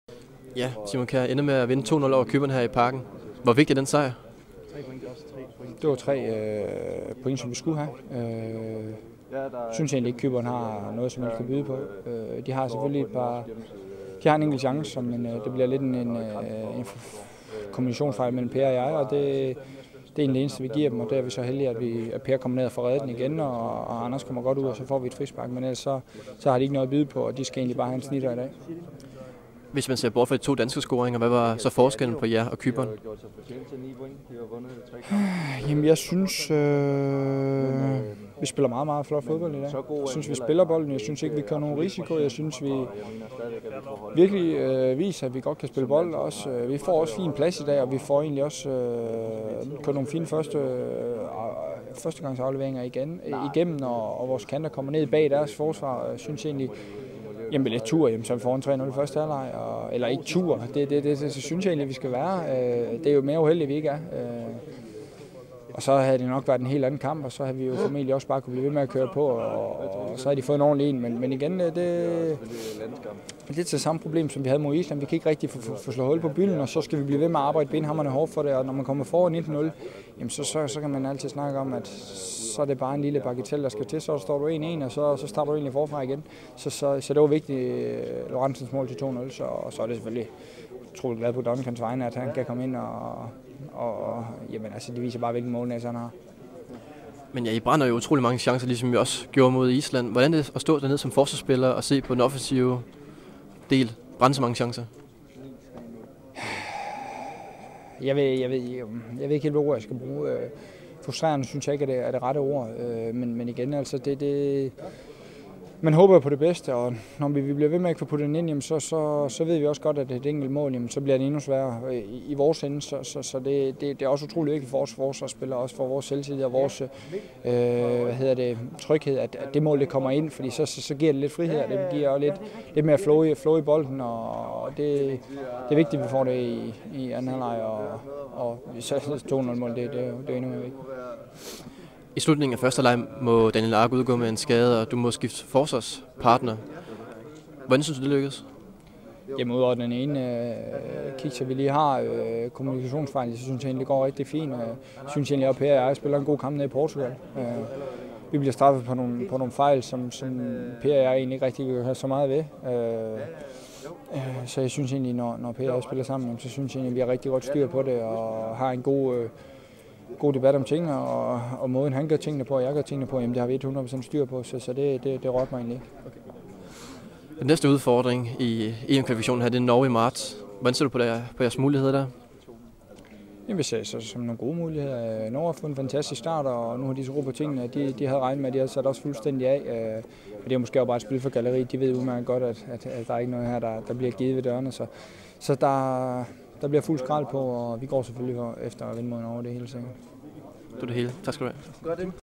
Post Match Interview with Simon Kjaer
An interview with Simon Kjaer after the game against Cyprus on Oct. 12, '10. Enjoy!